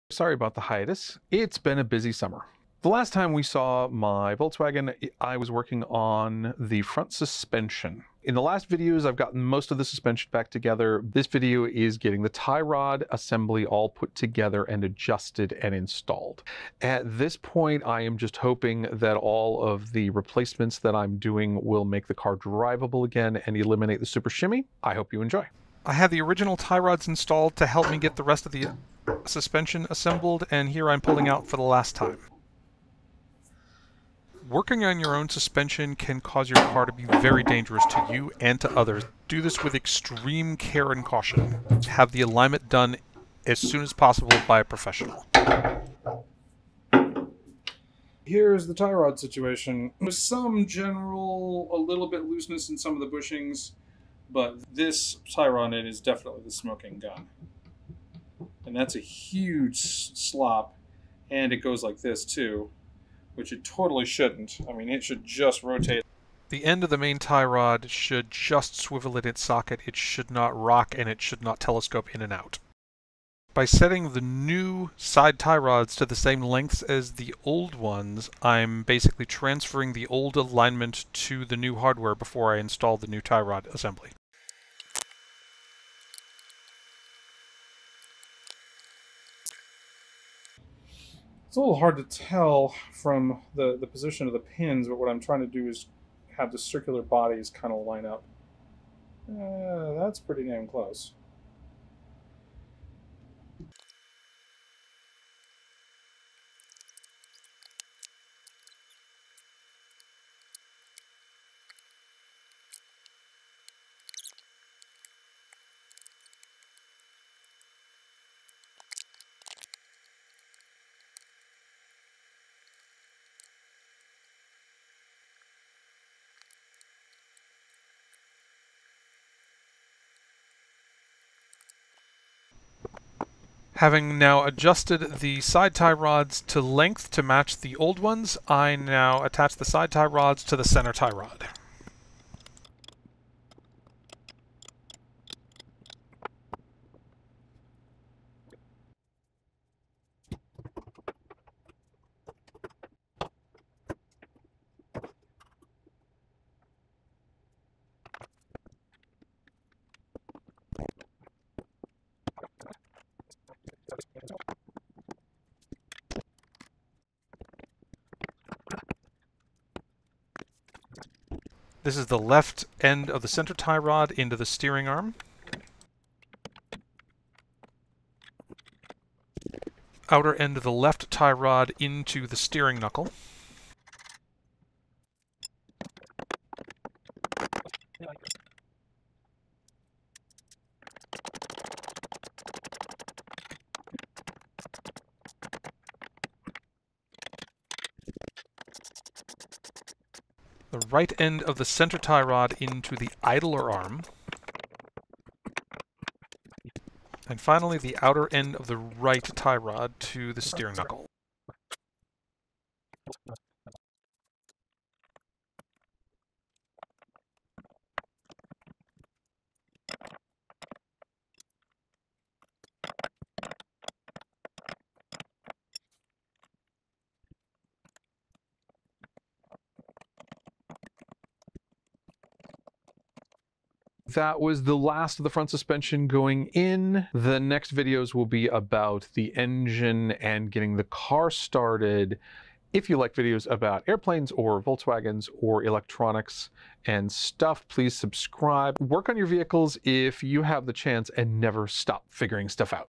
Tie rods into 1972 SuperBeetle
The last video about refurbishing the suspension.  Now on to the engine to get the car going in 2021.